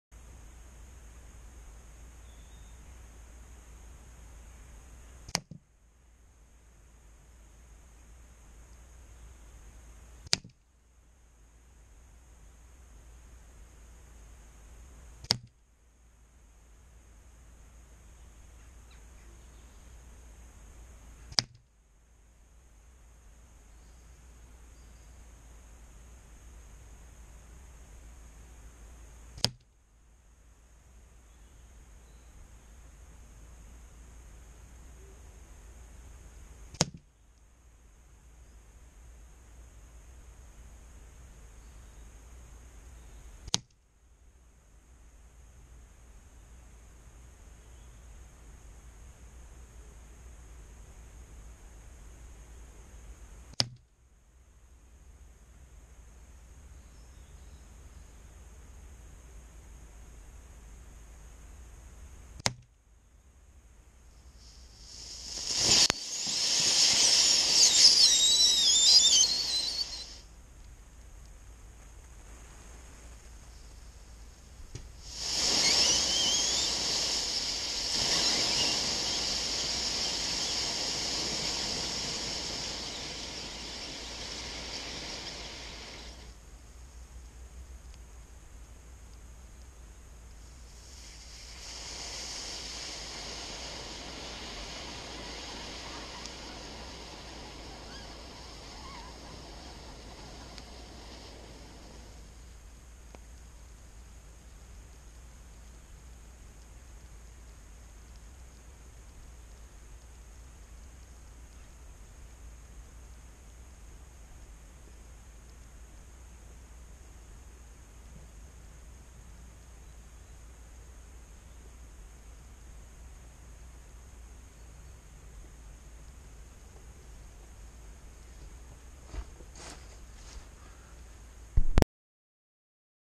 Disposing of Lipo's with Weihrauch HW100kt .177
Little disappointed they didn't all smoke but on closer inspection afterwards i was wedging the pellets between the cells and just denting them